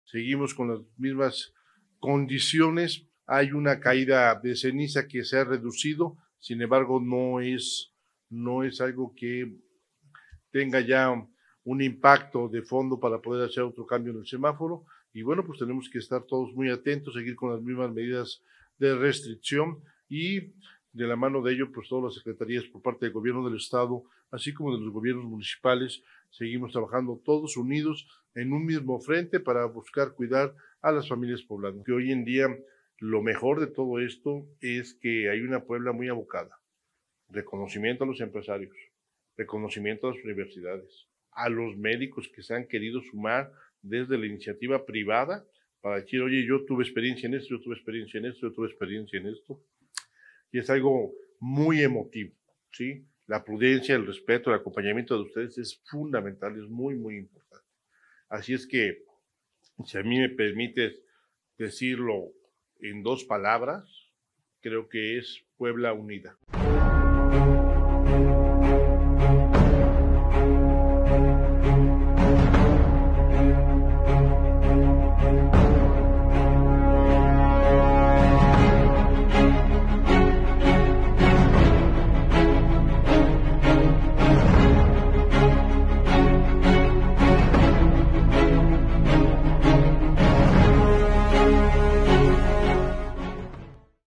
Ante actividad del Popocatépetl, Puebla está unida y fuerte
Ante actividad del Popocatépetl, Puebla está unida y fuerte: Sergio Salomón
-El titular del Ejecutivo reconoció a los diferentes sectores de la sociedad por contribuir con la autoridad para garantizar la seguridad y salud de las y los poblanos
-Recalcó que el Semáforo Volcánico está en Amarillo Fase 3 y que su gobierno actúa con responsabilidad